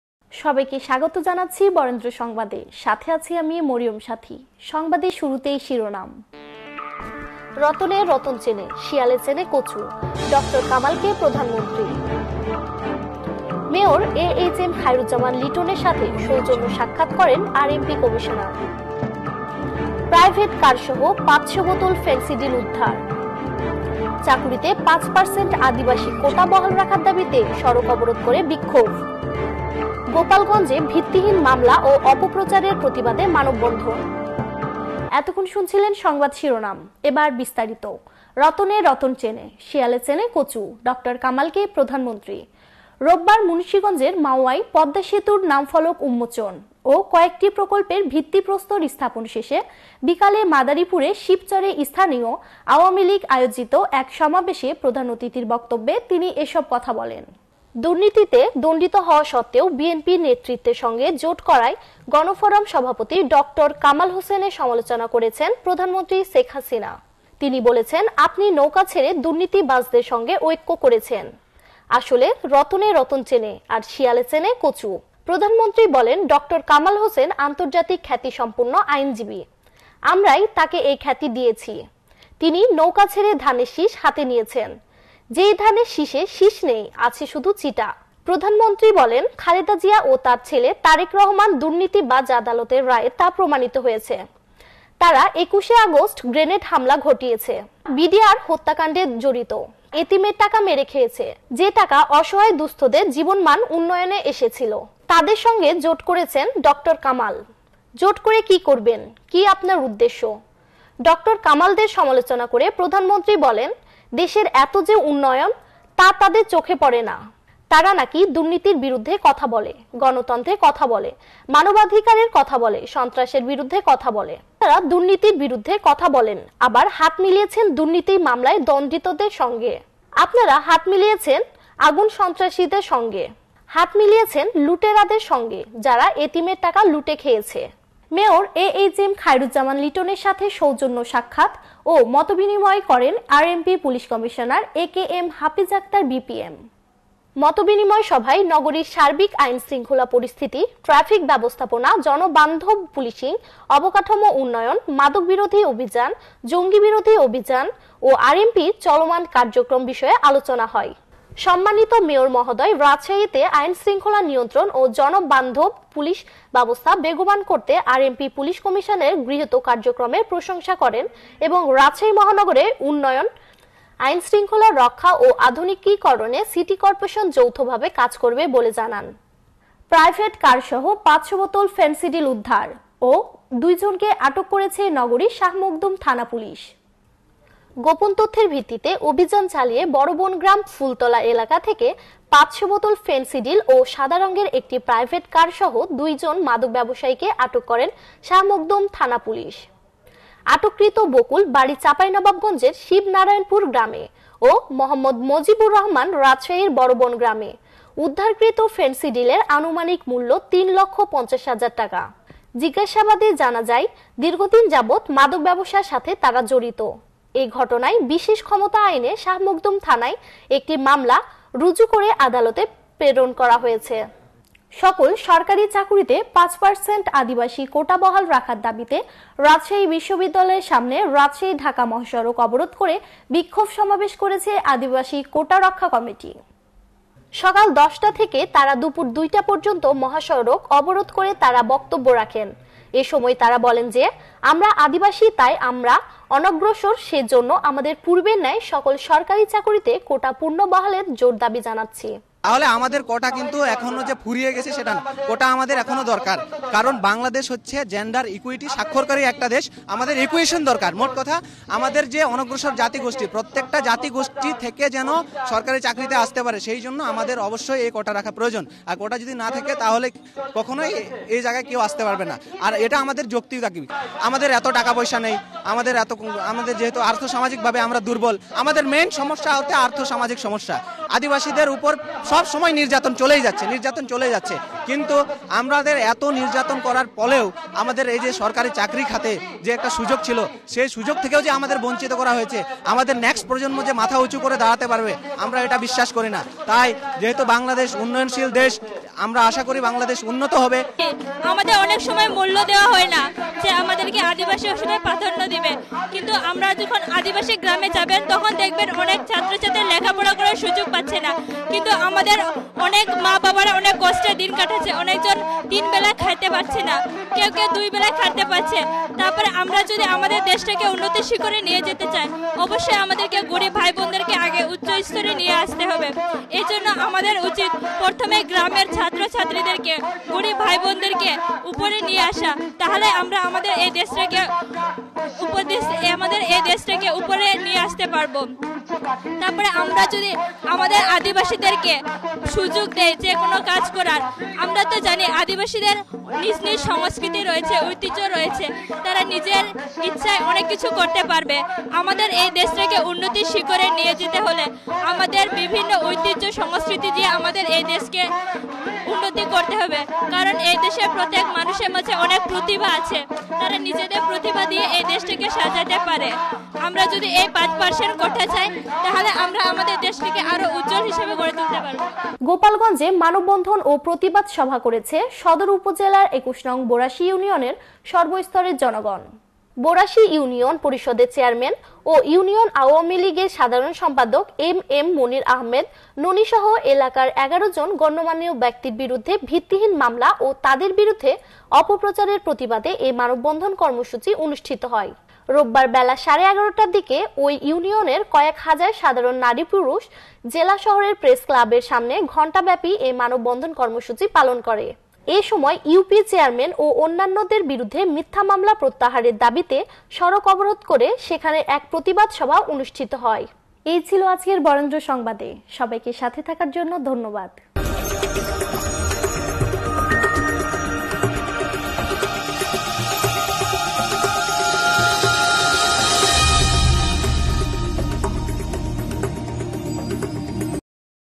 রতনে রতন চেনে শিয়ালে চেনে কচু ড. কামালকে...প্রধানমন্ত্রী
মেয়র এ এইচ এম খায়রুজ্জামান লিটন এর সাথে সৌজন্য সাক্ষাত করেন আরএমপি কমিশনার
প্রাইভেট কারসহ ৫০০ বোতল ফেন্সিডিল উদ্ধার
-চাকুরীতে ৫% আদিবাসী কোটা বহাল রাখার দাবীতে সড়ক অবরোধ করে বিক্ষোভ
গোপালগঞ্জে ভিত্তিহীন মামলা ও অপপ্রচারের প্রতিবাদে  মানববন্ধন